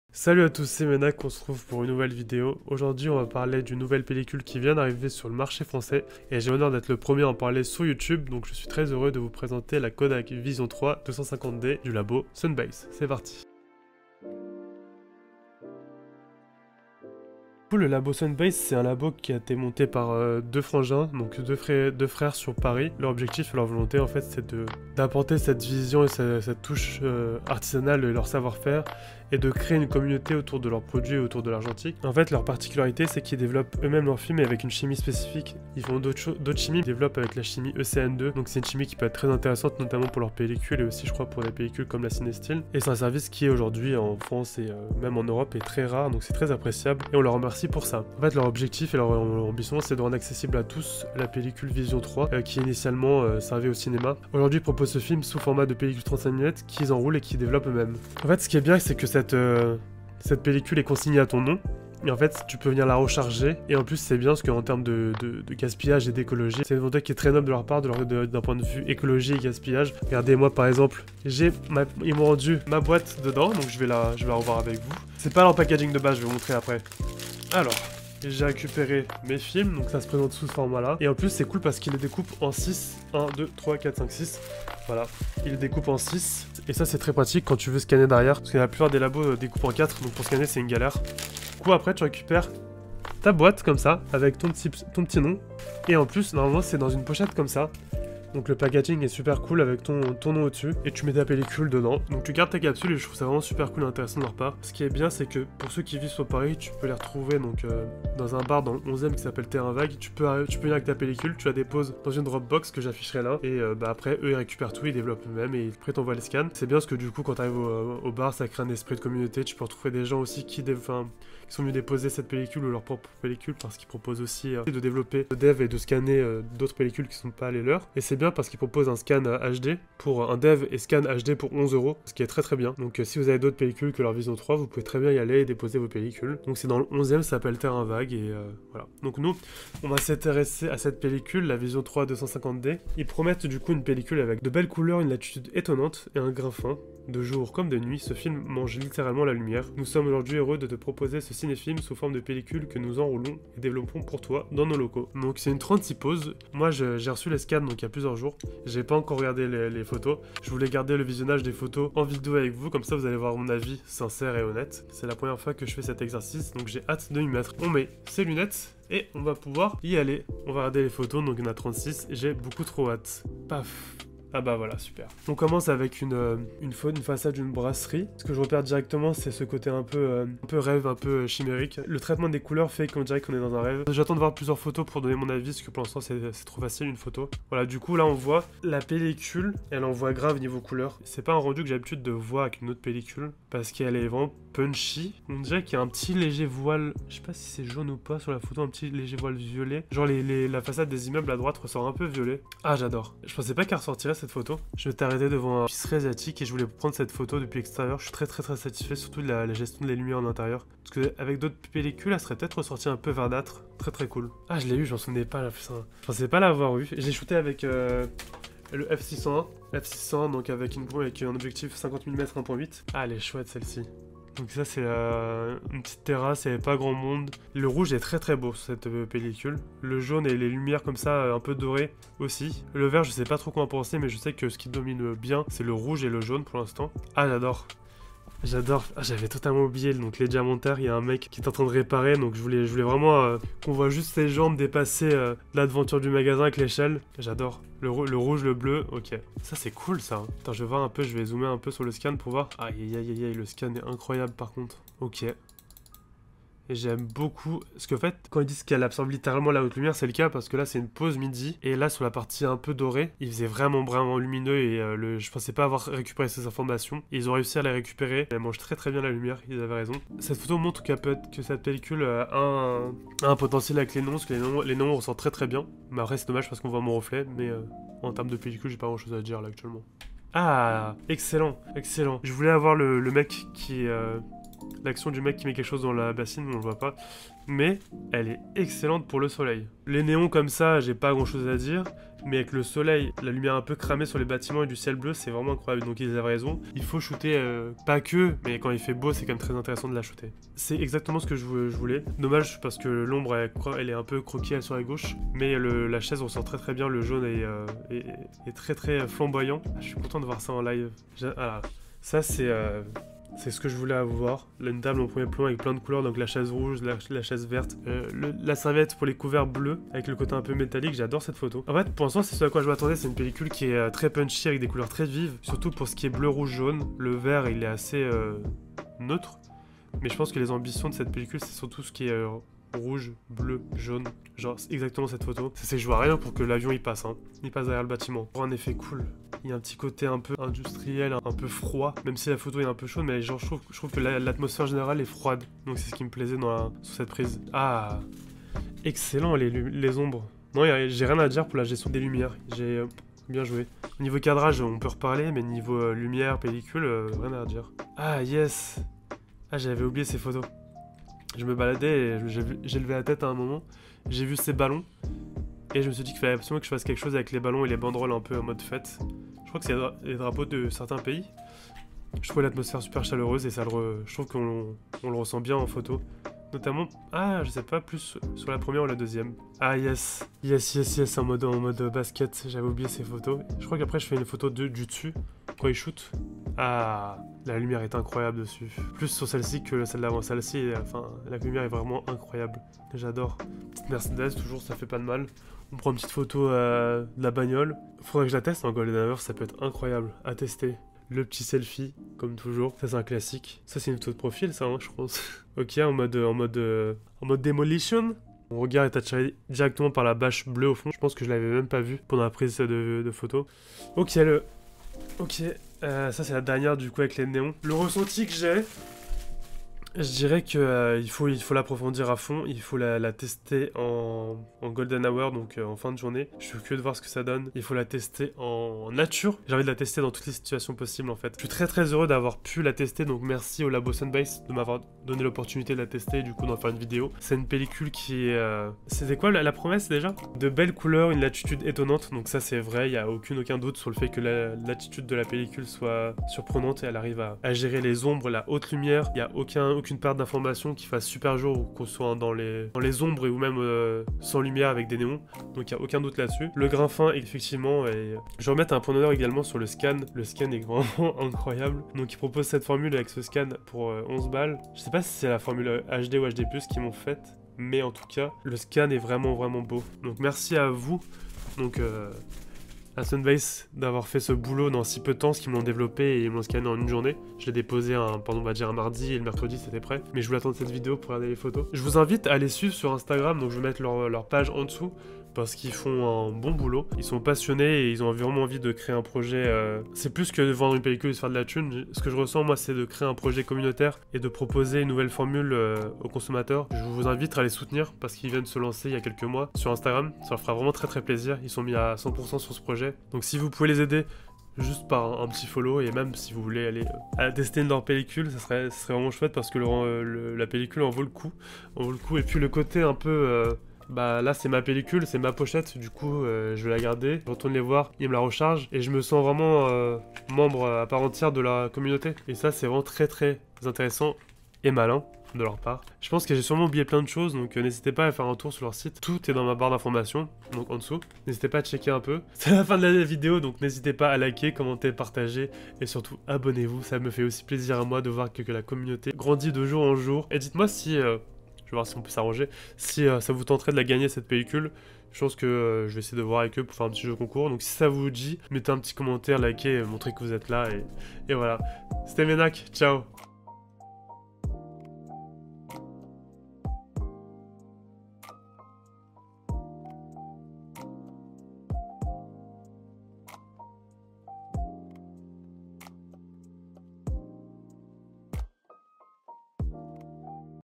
KODAK VISION 3 250D : La FUTURE pellicule couleur ? (Sunbath)
La KODAK VISION 3 250D : la nouvelle pellicule couleur pour nous apporter de la fraîcheur et du punch !